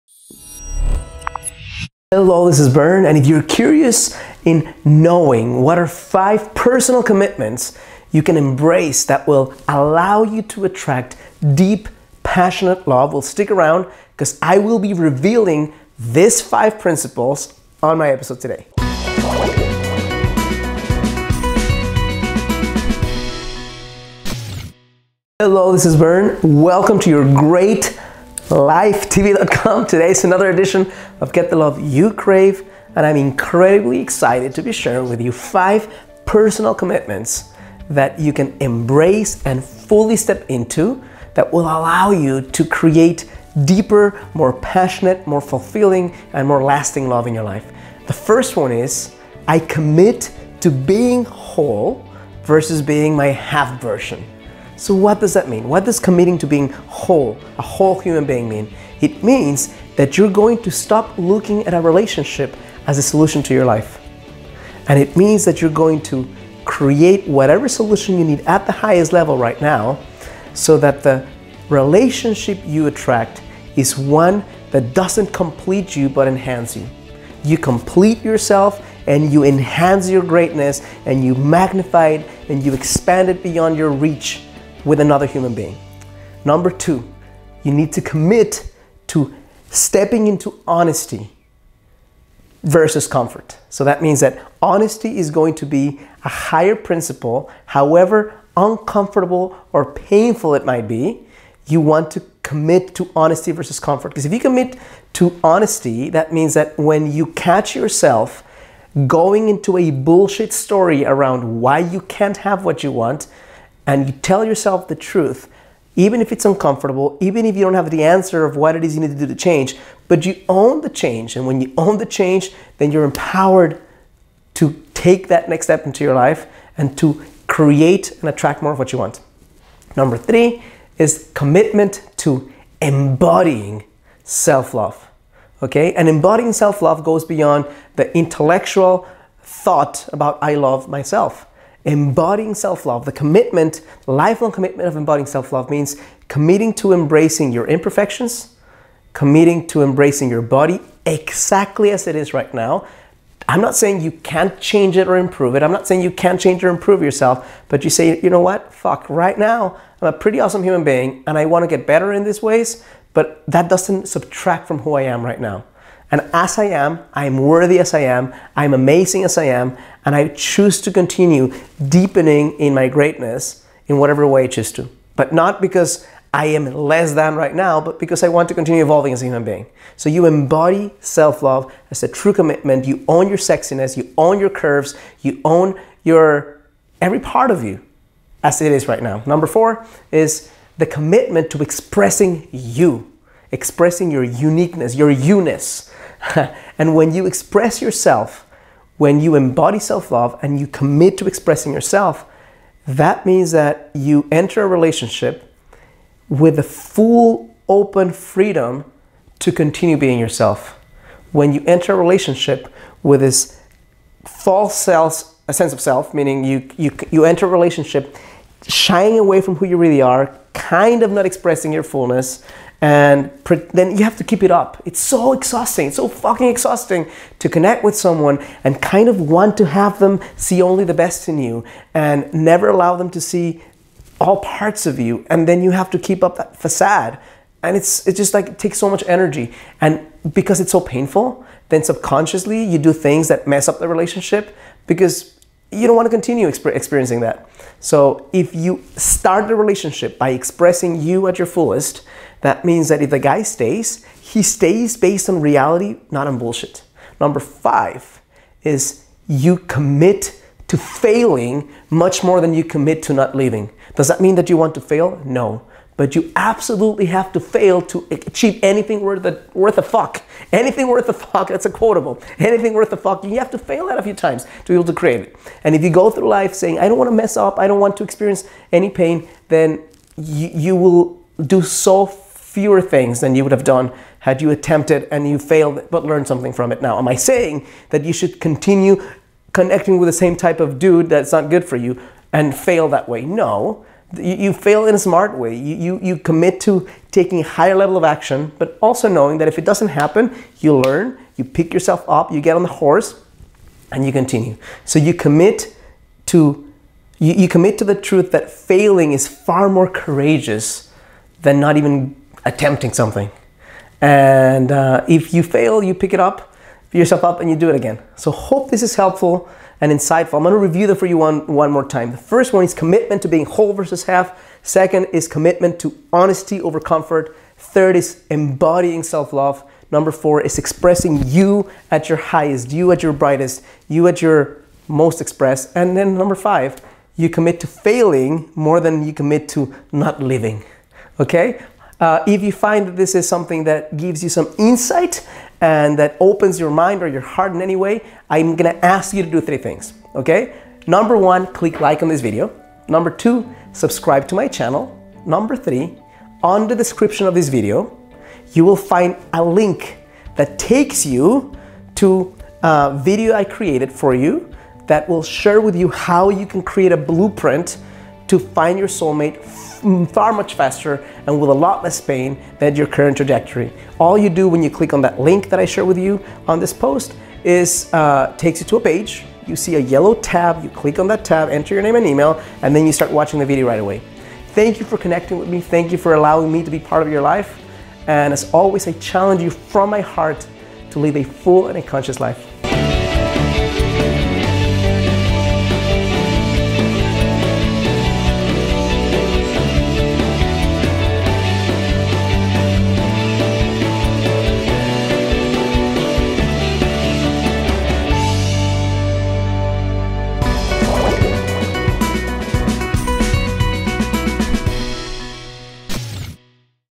5 Commitments To Experience Deeper Love
Today's episode highlights 5 commitments that can make your finding passionate love more real.